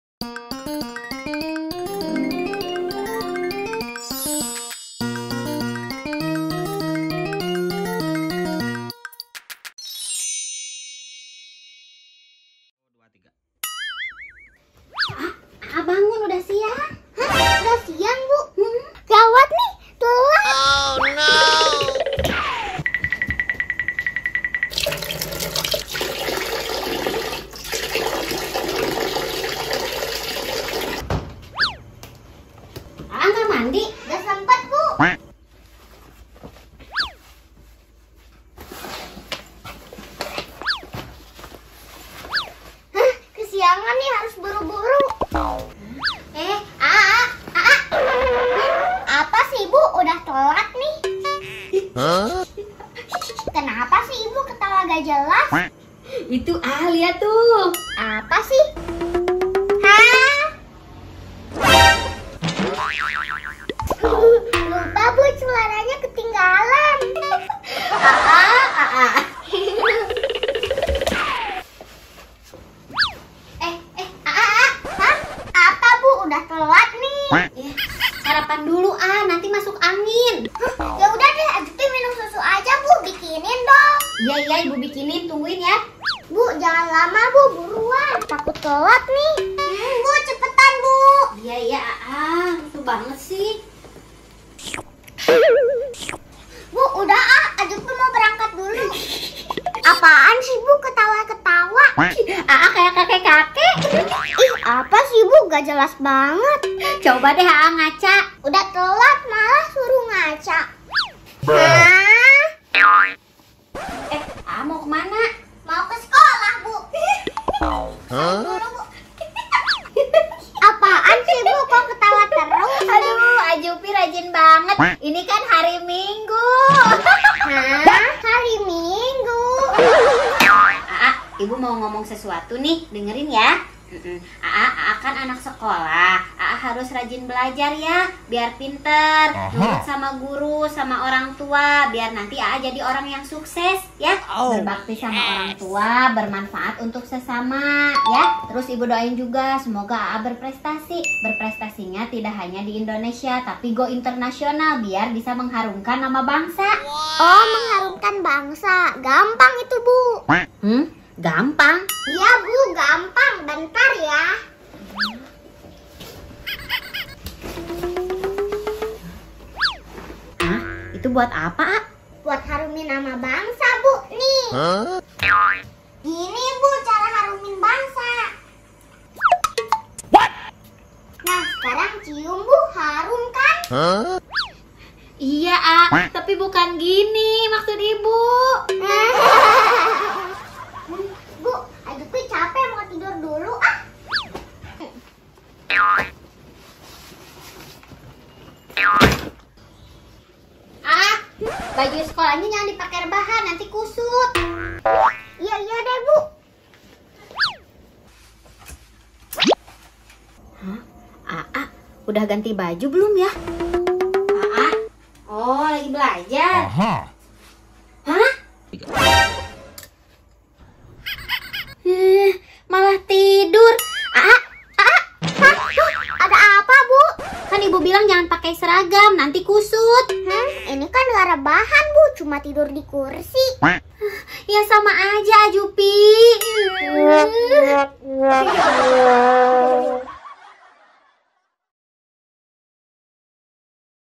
Anak Sekolah Kocak ! Azufi Lupa Pakai Celana Sekolah Karena Bangun Tidurnya Kesiangan
Ceritanya azufi bangun tidurnya kesiangan ... jadinya azufi rusuh untuk mempersiapkan segala keperluan sekolah ...  sampai-sampai azufi lupa pakai celana sekolah ... kocak banget anak sekolah ini